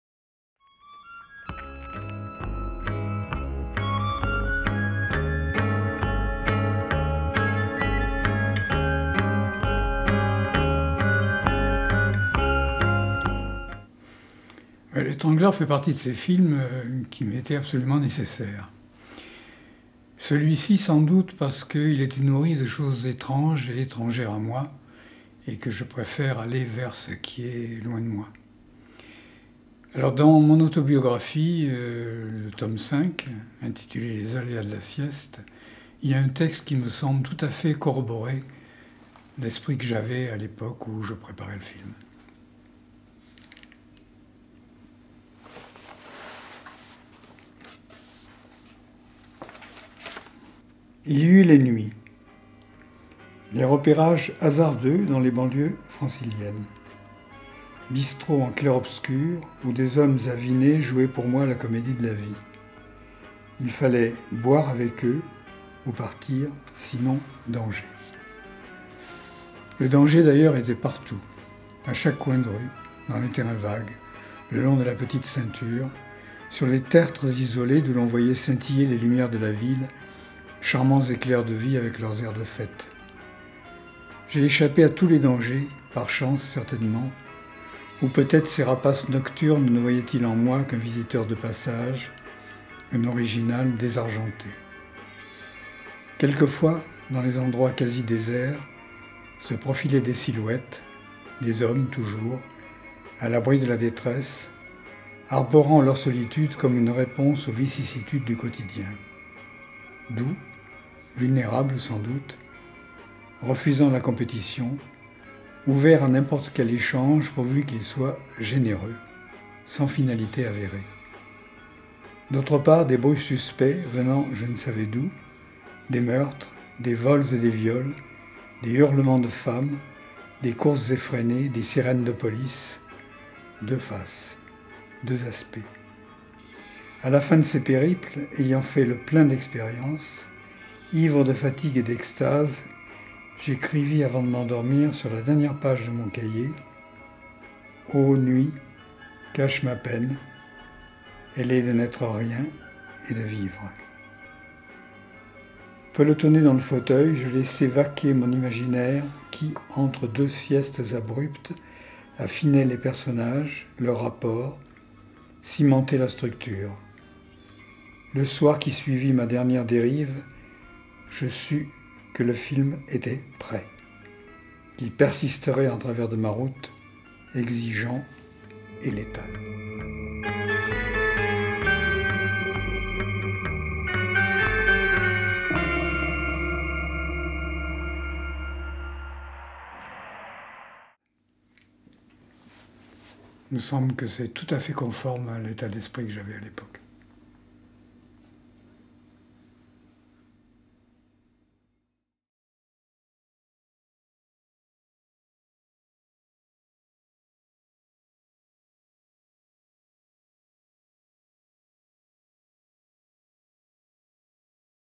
Paul Vecchiali GENESE de L'Etrangleur
L'Etrangleur de Paul Vecchiali - 1975 - Extrait des Bonus DVD - La Vie est Belle éditions. Paul Vecchiali nous évoque la genèse du film. Synopsis : Enfant, Émile croise, lors d'une fugue nocturne, un criminel qui étrangle sous ses yeux une femme en pleurs. Marqué à vie, il s'essaie, aux abords de la trentaine, de faire revivre cet instant « précieux ». Émile amasse ainsi dans la journée les renseignements nécessaires à l'accomplissement de ce qu'il ne croit pas être un crime.